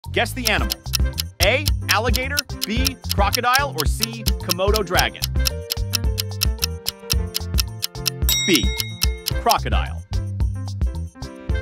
Guess The Animal Quiz Challenge Alligator Crocodile
Hop into the captivating world of amphibians with Quiz Row's "Crocodile and Alligator Quiz"! 🐸🌿 Dive into the riveting realm of these fascinating creatures as we challenge you to guess the various species of frogs and toads from around the world.

Get ready to test your knowledge and observation skills as we present intriguing facts, captivating images, and puzzling questions about these croaking critters. From colorful tree frogs to camouflaged toads, there's so much to discover in the diverse world of amphibians!

Whether you're an amphibian aficionado or simply curious about the creatures that inhabit our planet, this quiz is bound to captivate and educate. Join us on Quiz Row for a ribbiting adventure through the enchanting realm of frogs and toads!